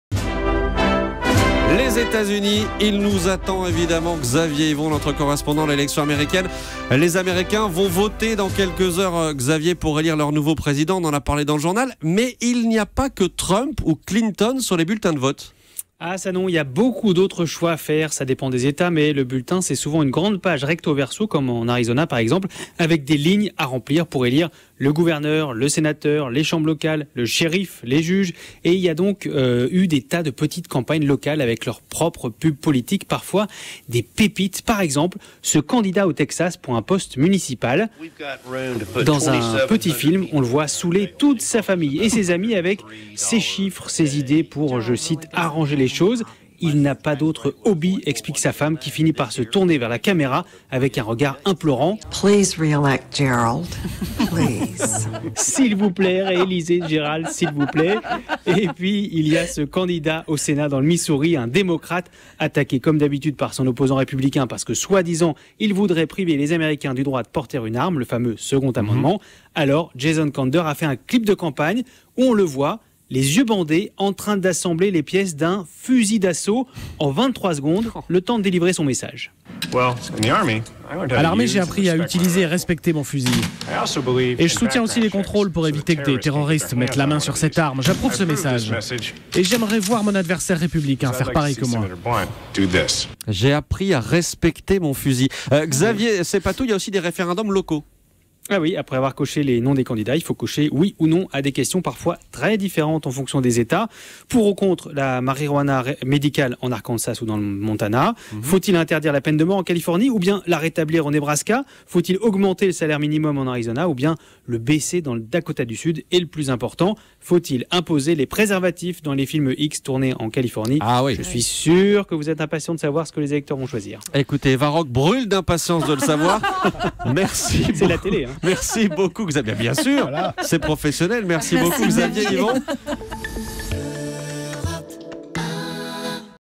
Élection américaine : il n'y a pas que Trump et Clinton sur les bulletins !
À quelques heures de l'ouverture des bureaux de vote, il est important de rappeler que les électeurs ne voteront pas uniquement pour Clinton ou Trump.